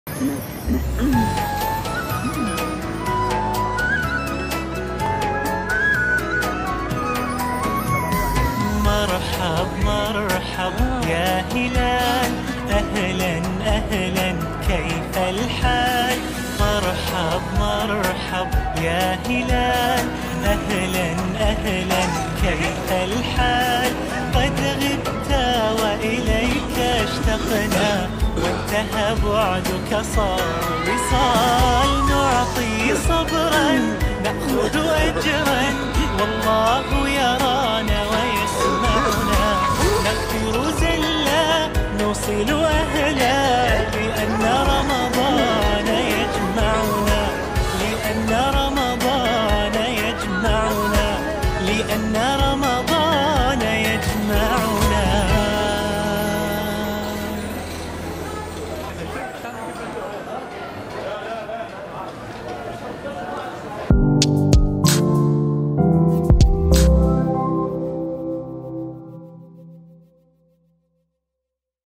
استعدادات أهالي الرقة لاستقبال شهر رمضان
ازدحام أسواق مدينة الرقة بالأهالي استعدادًا لاستقبال شهر